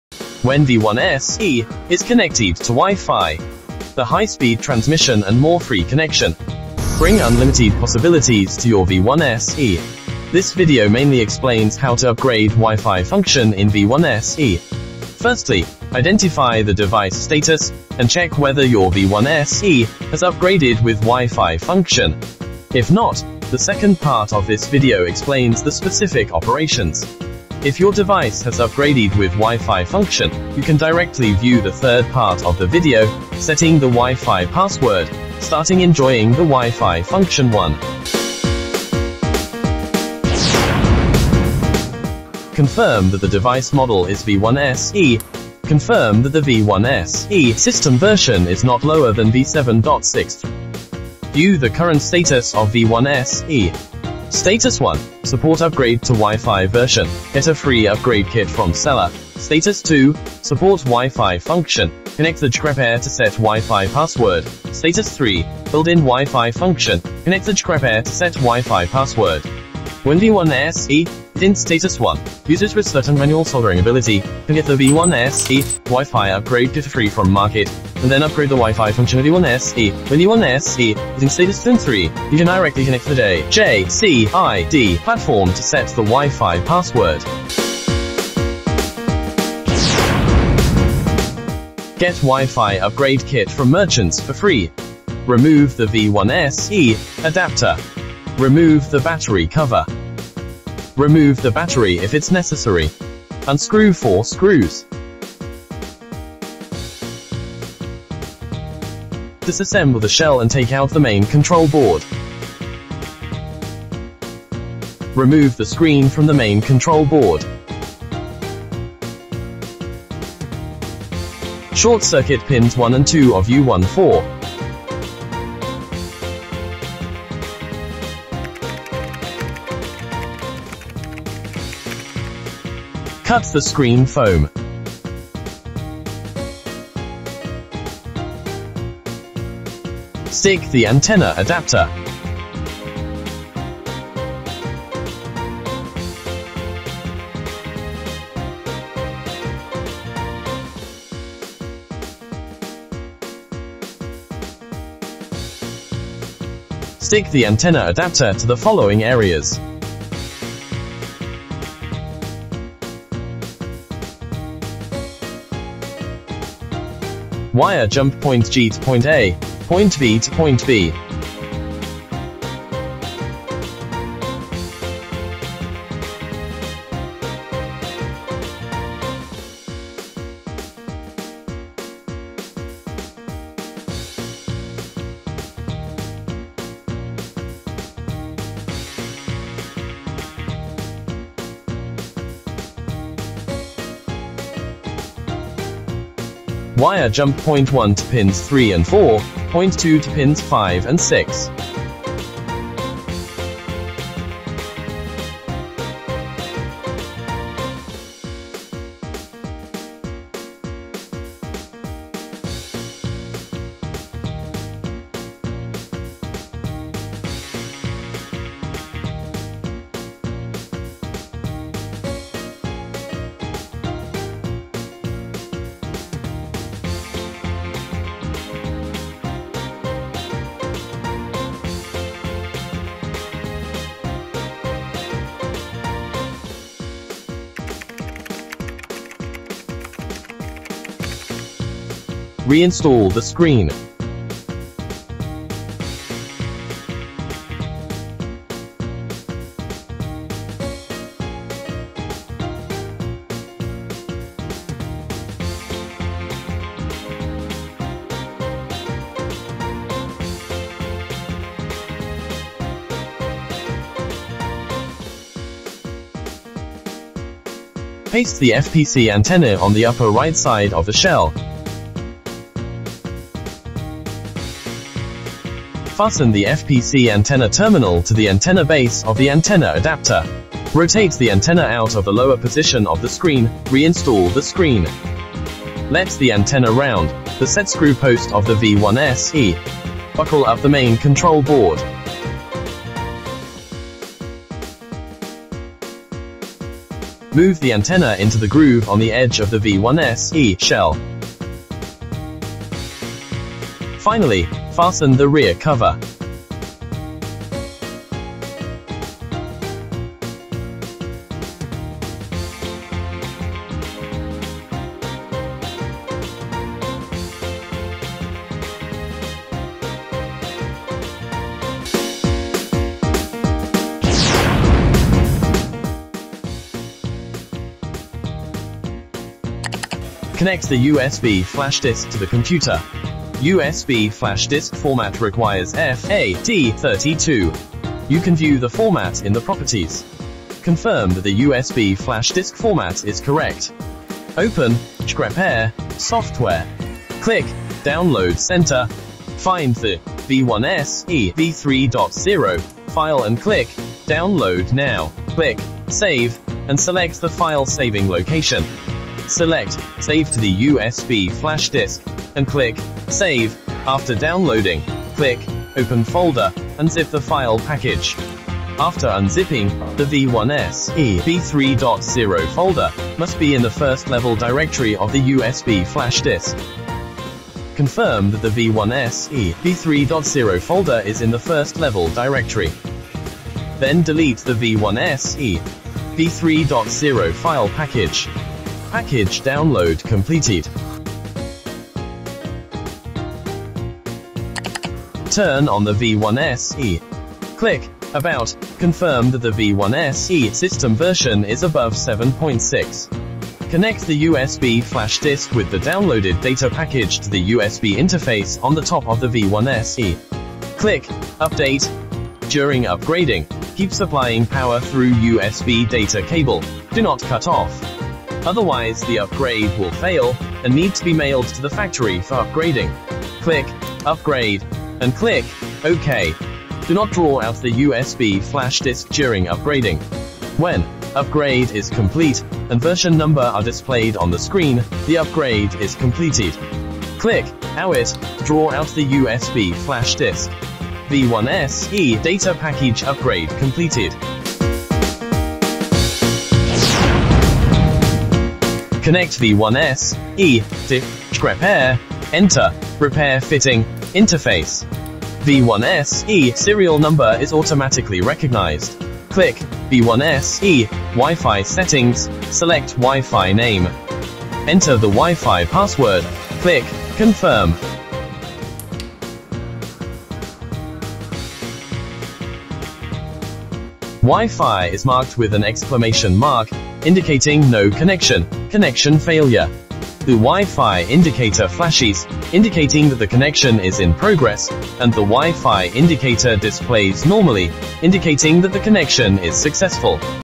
How to Upgrade JC VISE Add WIFI function丨Operation Teaching
How to upgrade WiFi function in V1SE
1. Identify device status
2. Methods of upgrading WiFi Function
Step 1: Install WiFi Antenna
Step 2: USB Flash Disk upgrade firmware
3. Set WiFi Password Start Enjoying WiFi Function